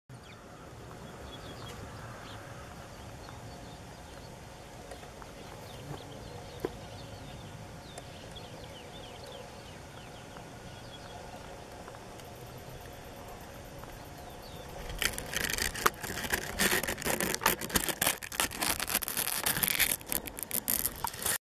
Lesvos Bush Cricket (Poecilimon mytelensis)
Endemic to Lesbos. Filmed at Aghios Ioannis on 4th May 2014 with a Canon PowerShot SX50 HS.